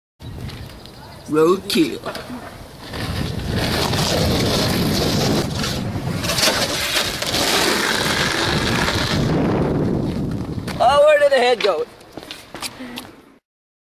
clip of the day #1
kickflip over road kill